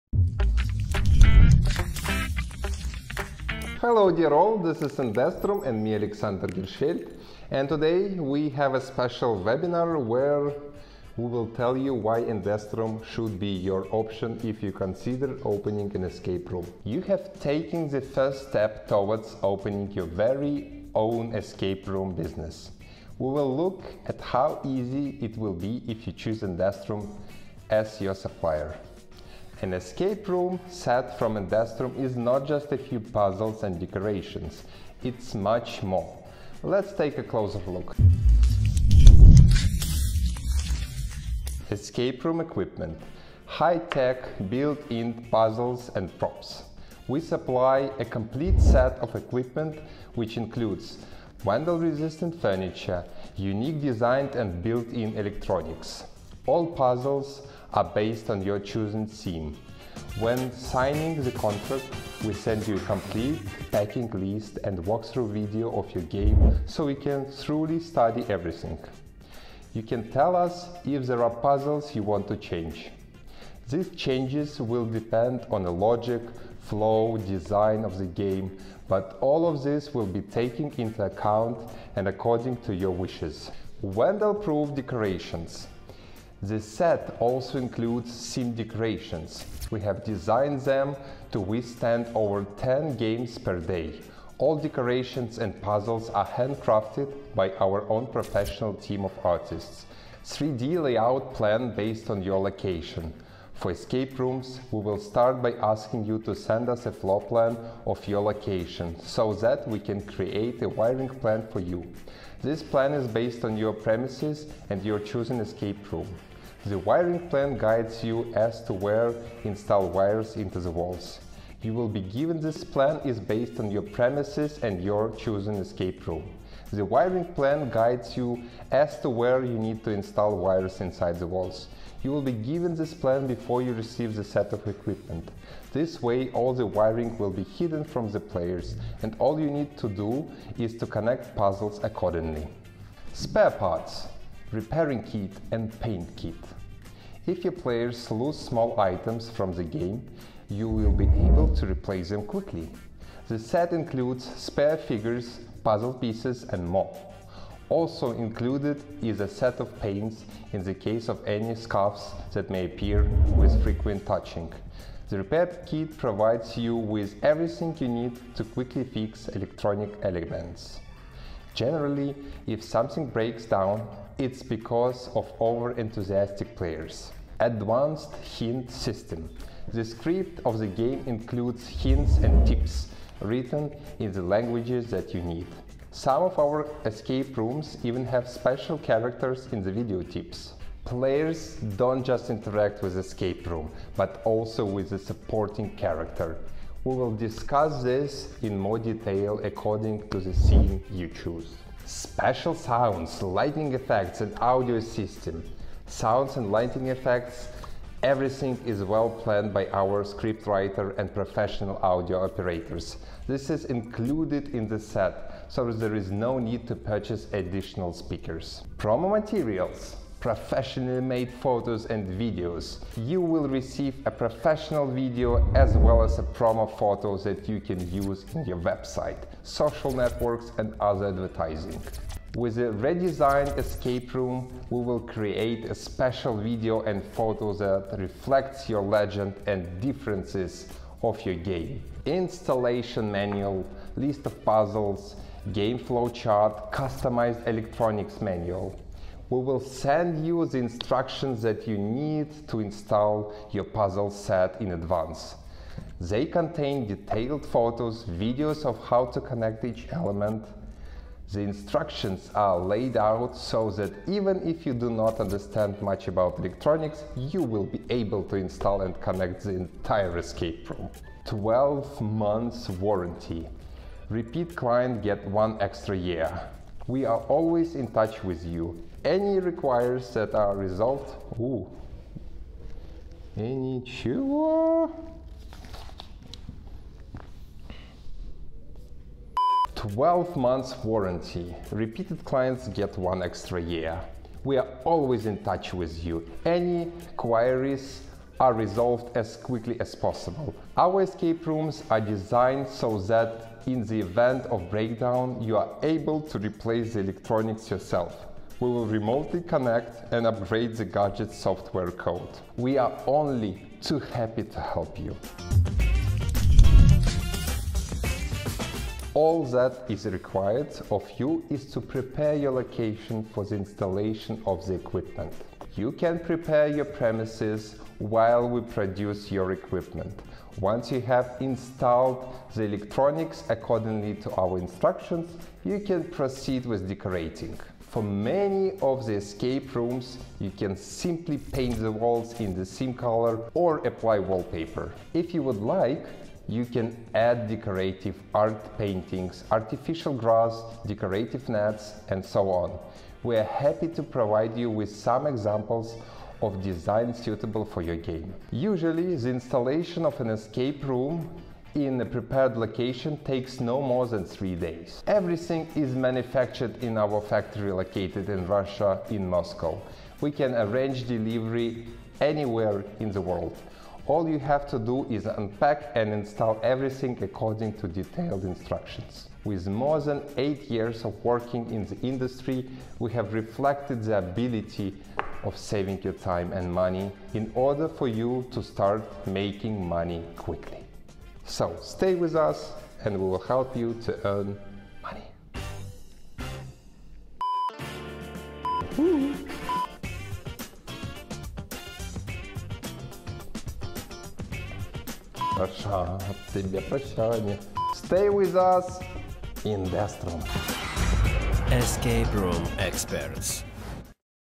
WHAT YOU GET AND WHAT YOU NEED TO PREPARE?
You have taken the first step towards opening your very own escape room business. We will look at how easy it will be, if you choose INDESTROOM as your supplier.

An escape room set from INDESTROOM, is not just a few puzzles and decorations, it’s much more. Let’s take a closer look!

In this webinar, we will show you exactly what you get by purchasing any product from us.